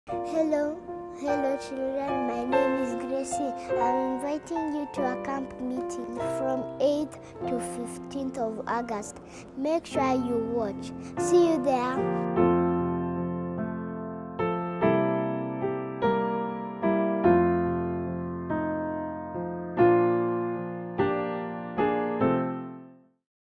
Camp Meeting 2020 Children Ministry Invite
The Seventh Day Adventist Church Nairobi Central SDA Church Children Ministry department would like to invite the Children for our annual Camp Meeting Scheduled for the 8th-15th of August 2020. We are going to have a Virtual program and all the children are invited to attend. God bless you.